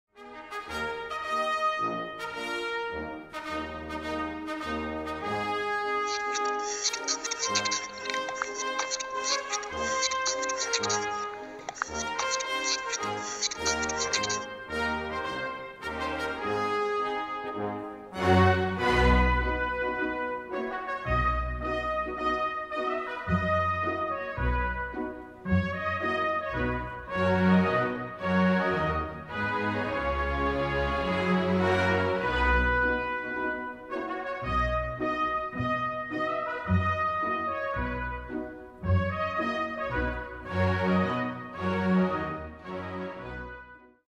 Auerhahn Stresstest
Wie "Männer" ihr Revier gegen Eindringlinge verteidigen.